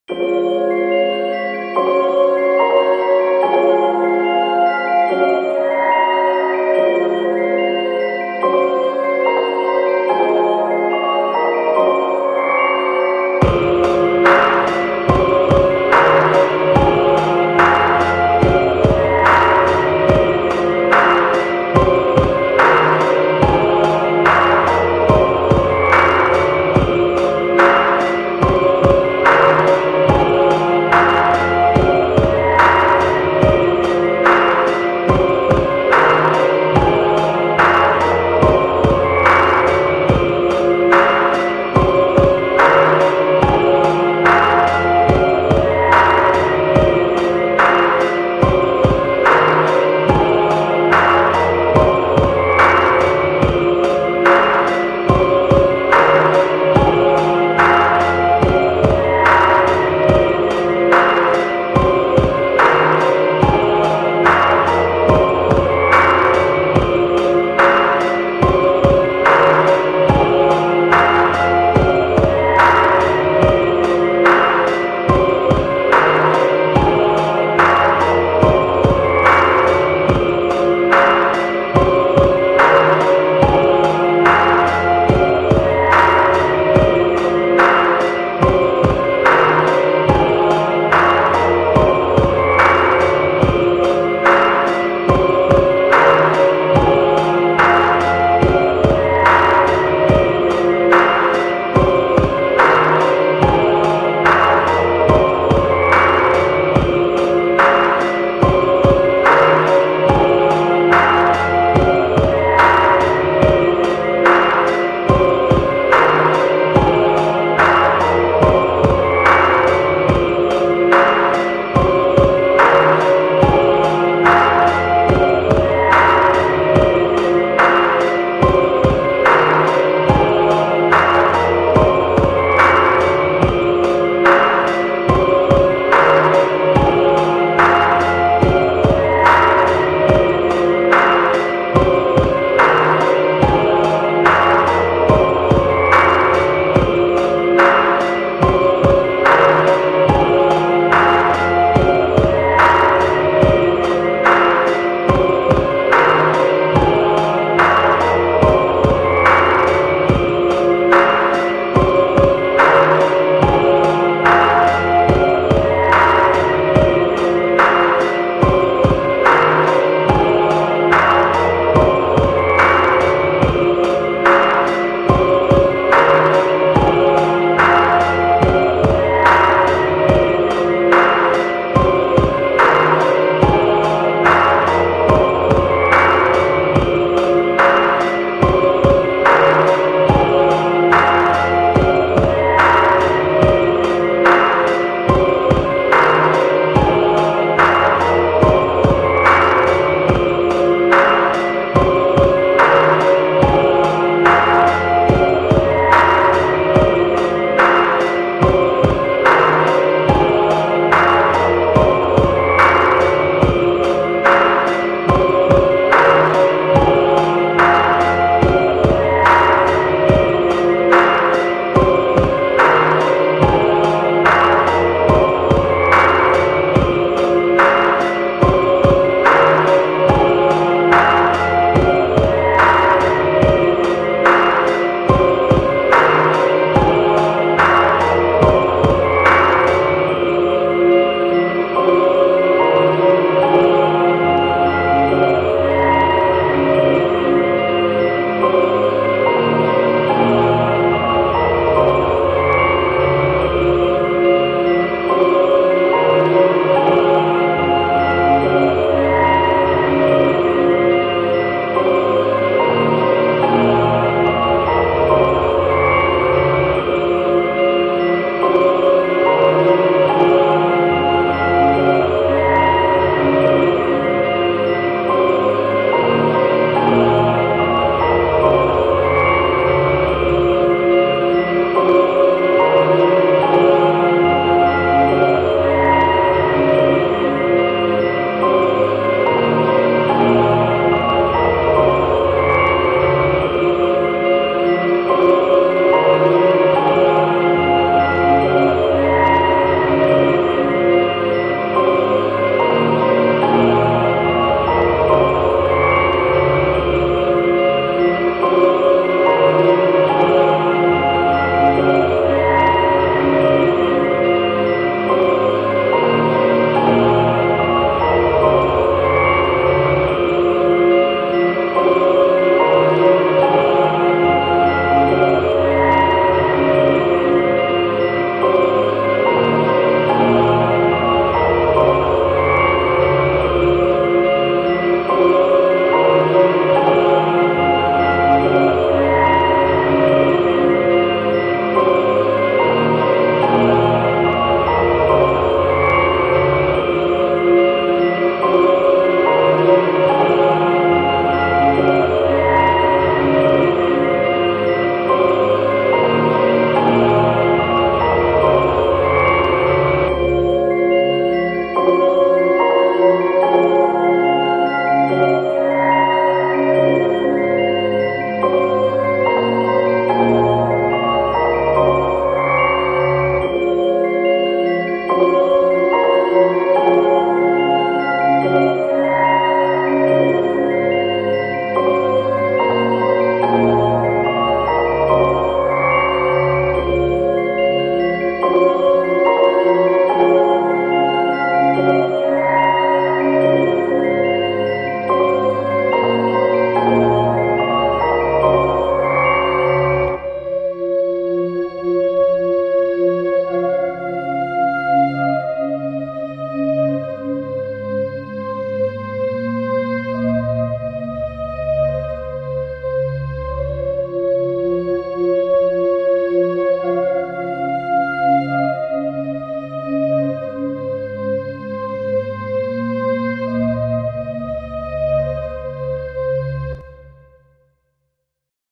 .13}{ECHOES WITHIN }{THE WHITE QUEEN'S GRAVE}
{ECHOES WITHIN}{THE WHITE QUEENS GRAVE}Is About The White Queen coming back from the dead for one last dance with Alice and In The End Alice puts the White Queen Back in her own Grave forever.